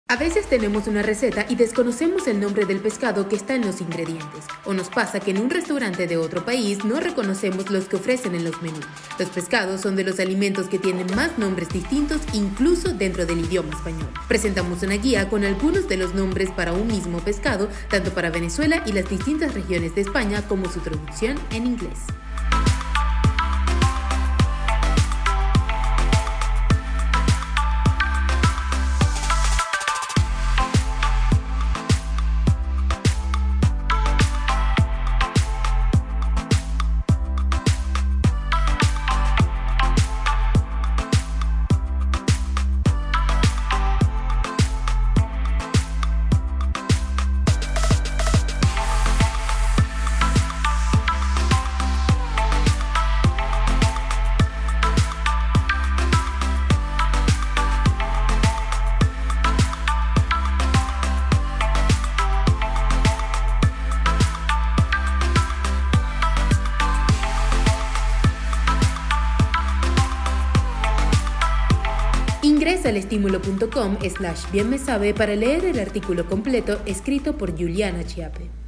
10 pescados comunes y sus nombres en español y en inglés
Los pescados son de los alimentos que tienen más nombres distintos, incluso dentro del idioma español.
Aquí te ofrecemos una guía de los distintos nombres que tienen los pescados de uso común, para poder reconocerlos en otros países.